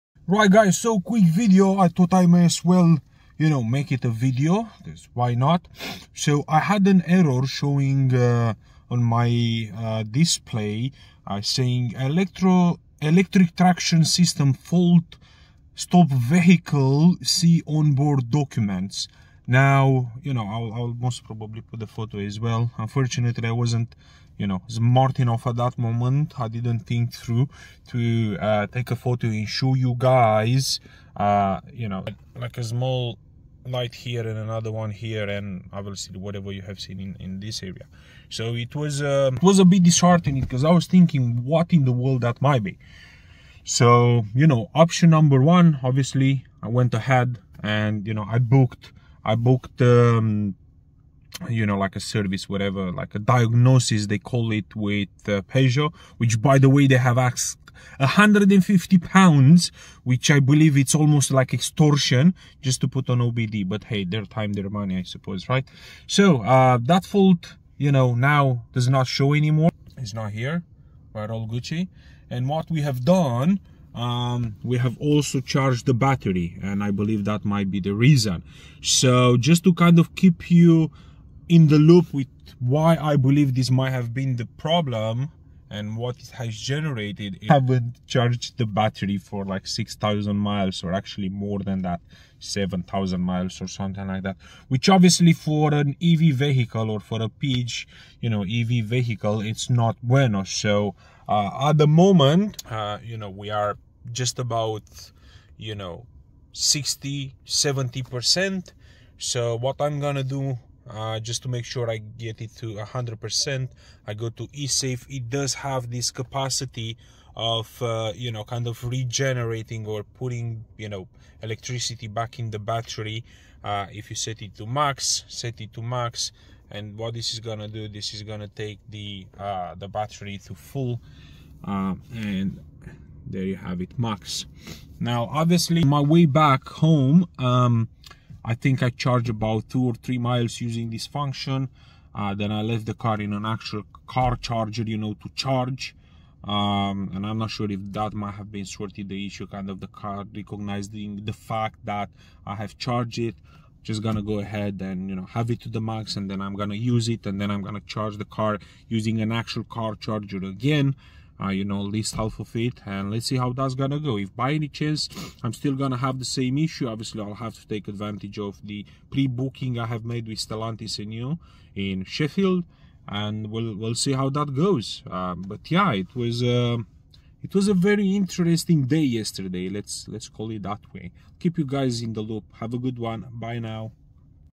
Electric Traction System Fault: Stop Vehicle and See Onboard Documents
Encountering an "Electric Traction System Fault" warning in your vehicle? This can be a critical issue that requires immediate attention. In this video, I am addressing this warning and how I have worked around it. Did it work? We'll find out.